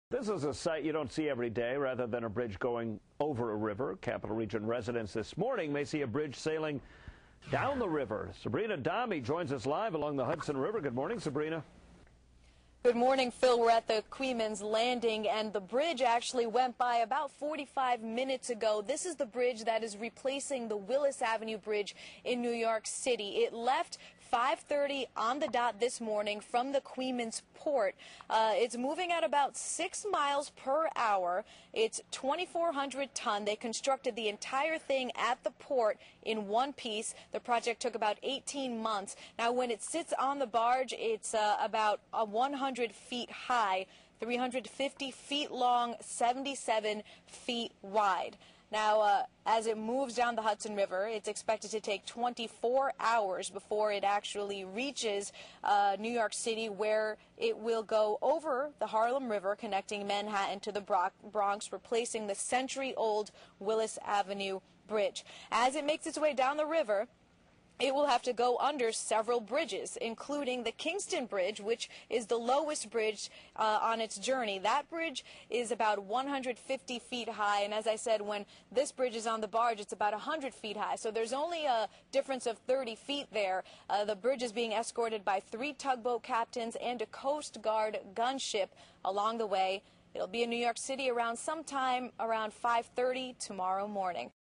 Bridge moving down Hudson River
Its 350 feet long, 65 feet high and 77 feet wide.&nbsp; The new Willis Avenue Bridge weighs 2,400 tons, and took 18 months to construct.&nbsp; The massive structure is now on a barge at the Coeymans Port.&nbsp; It took 400 wheels to move it from the staging area to the barge early this morning.